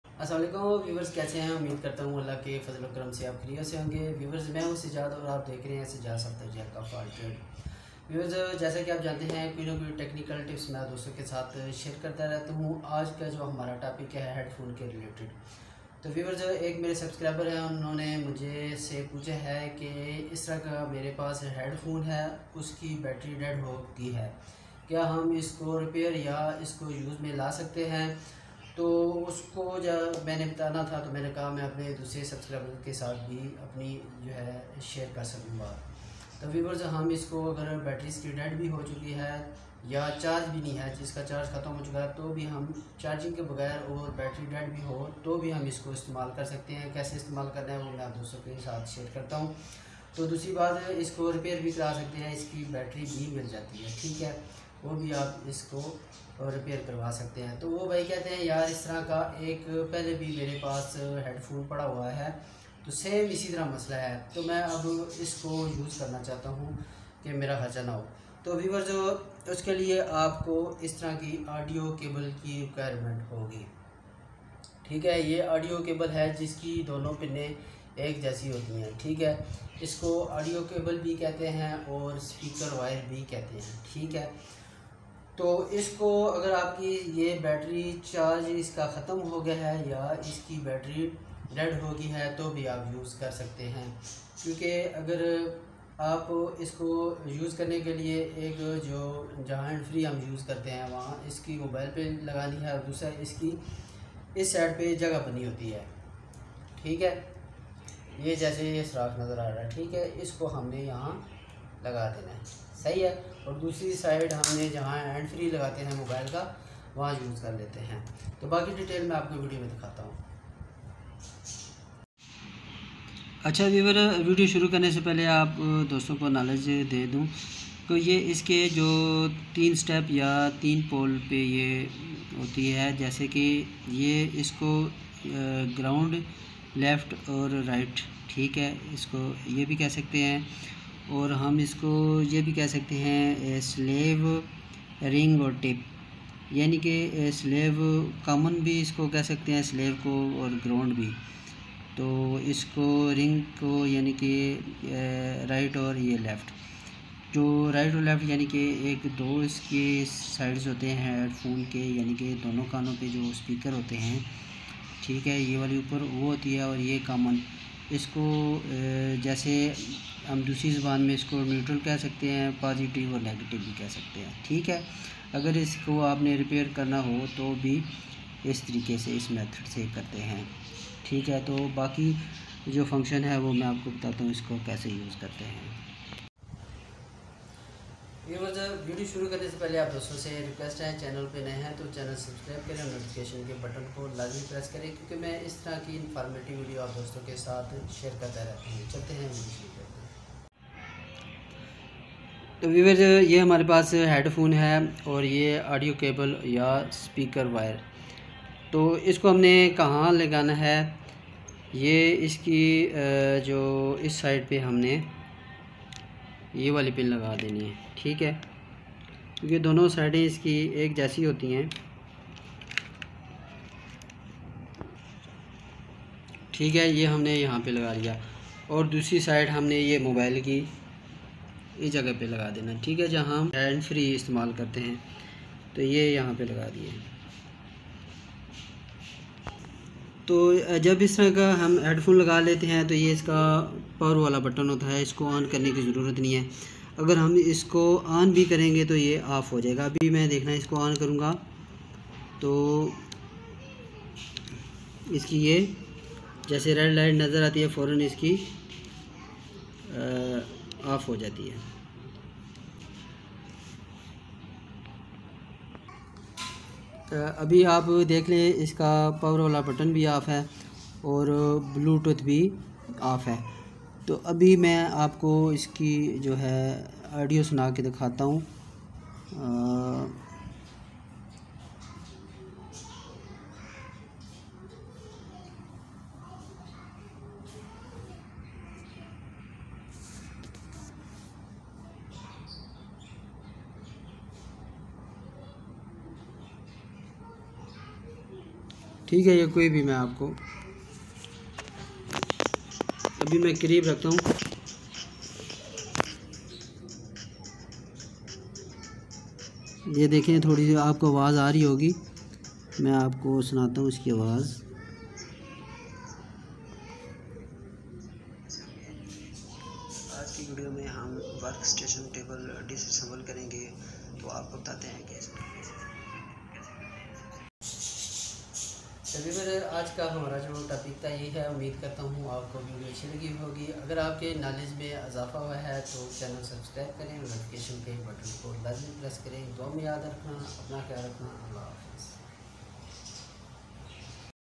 How To Connect Headphones Without Bluetooth | Bina Battery Ke Bluetooth Headphones Kaise Use Kare
How To Run Bluetooth Headphones Without Battery| Bina Battery Ke Bluetooth Headphones Kaise Use Kare| How To Connect Headphones Without Bluetooth

How To Assemble Stapler | How To Repair Stapler Machine